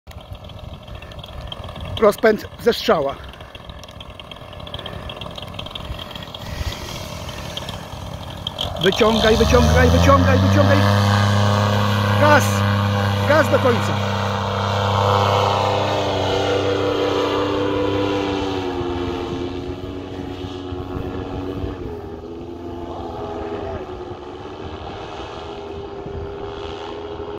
Start na paralotni Dudek Synthesis2 PPG Paramotor How to Launch Paragliding school
paralotnia Dudek Synthesis2 napęd JPX Black Devil.
How To Launch A Paramotor Launch